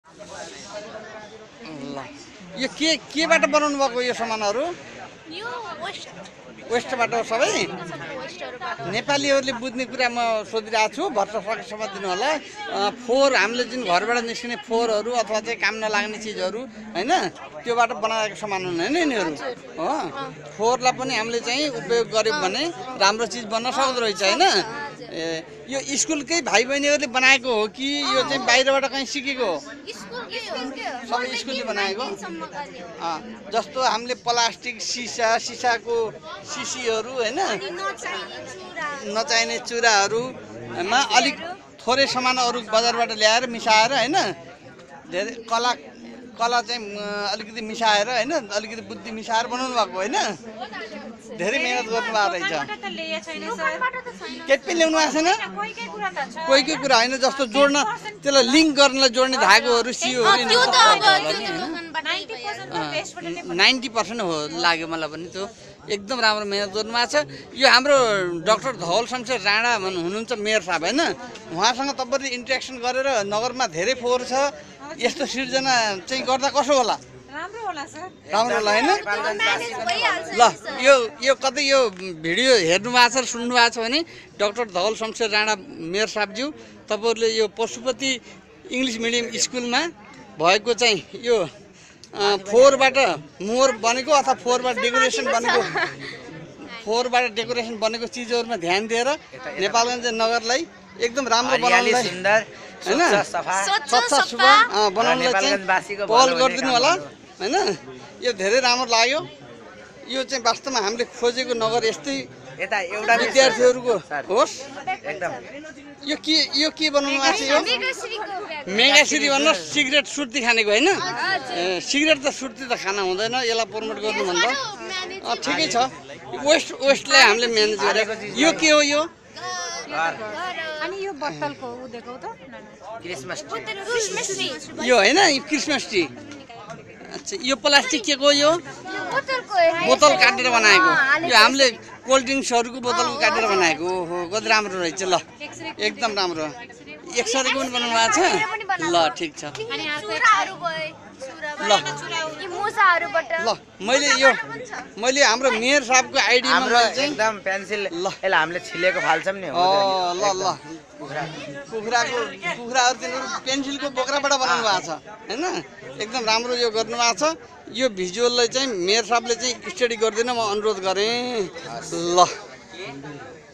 Waste management of Pashupati EMS Ranjha Nepalgunj
Waste material as cold drinks bottle, packets, papers etc used for better decorative show PCs and articles !